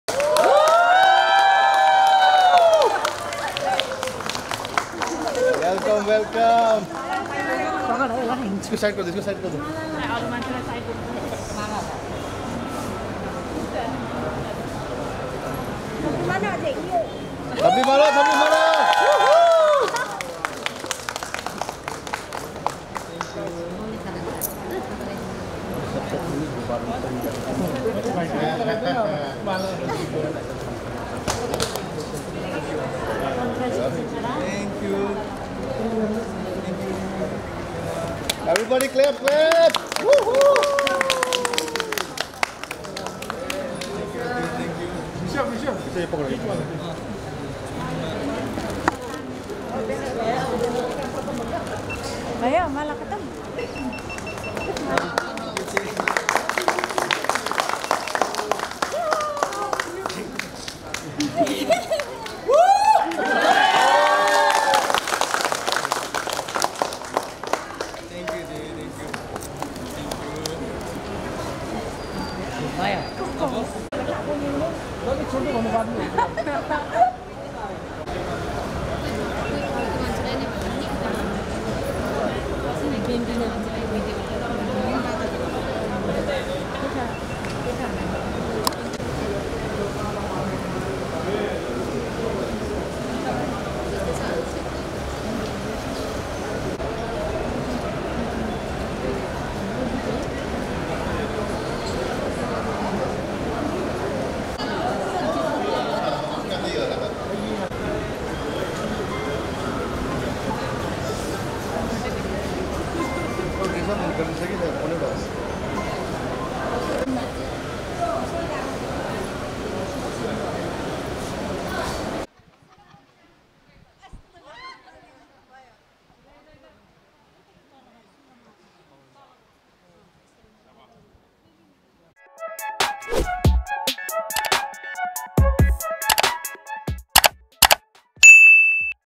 Grand Welcome for Rubaru Mr India 2023 Model International Reshab Chetri at Imphal airport
Hailing from Mantripukhri in Imphal West, Reshab Chetri represented Manipur in the highly  Rubaru Mr India 2023 competition. Besides being crowned with the 'Model International' title, Reshab Chetri also clinched the 'Best Runway Model' sub-title.